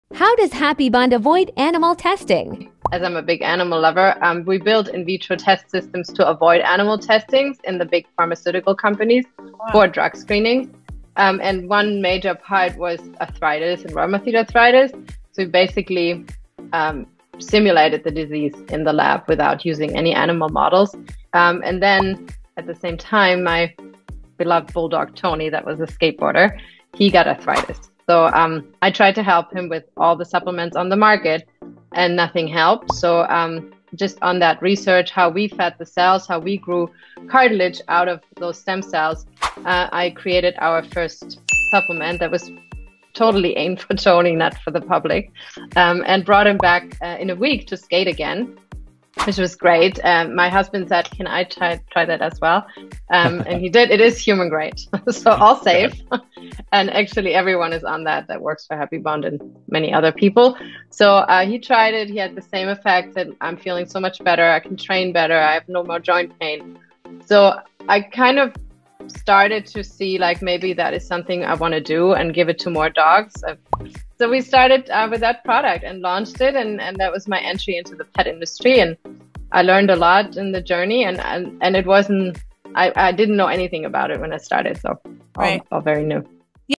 Skateboarding Dog Beats Arthritis Without Animal Testing! 🐶🛹
Discover the heartwarming story of Tony the skateboarding bulldog and the groundbreaking supplement that got him back on his board in just a week—without a single animal test!

Dive into this inspiring short to learn how HappyBond's founder revolutionized pet health care using cutting-edge in vitro tests, all sparked by love for her furry friend.

Witness the journey from lab breakthrough to life-changing product, now loved by dogs and humans alike.

If you're an animal lover seeking ethical, effective solutions for joint pain, this story of innovation, dedication, and skateboarding dogs is a must-watch.